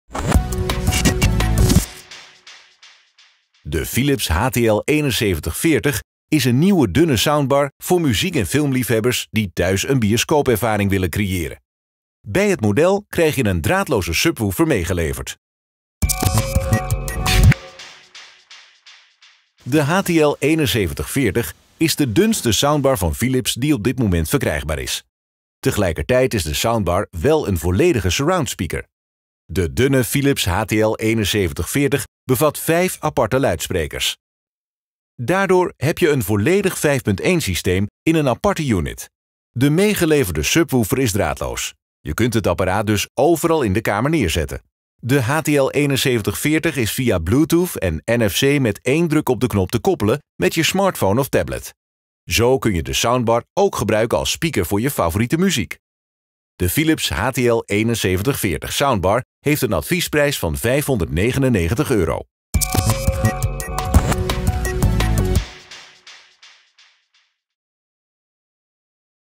Philips HTL7140 Soundbar - EverythingVideo
De Philips HTL7140 is een nieuwe dunne soundbar voor muziek- en filmliefhebbers die thuis een bioscoopervaring willen creëren.

Bij het model krijg je een draadloze subwoofer meegeleverd.

De HTL7140 is de dunste soundbar van Philips die op dit moment verkrijgbaar is. Tegelijkertijd is de soundbar wel een volledige surround speaker.
Daardoor heb je een volledig 5.1 systeem in slechts een aparte unit.
De meegeleverde subwoofer is draadloos. Je kunt het apparaat dus overal in de kamer neerzetten.

De Philips HTL7140 Soundbar heeft een adviesprijs van 599 euro.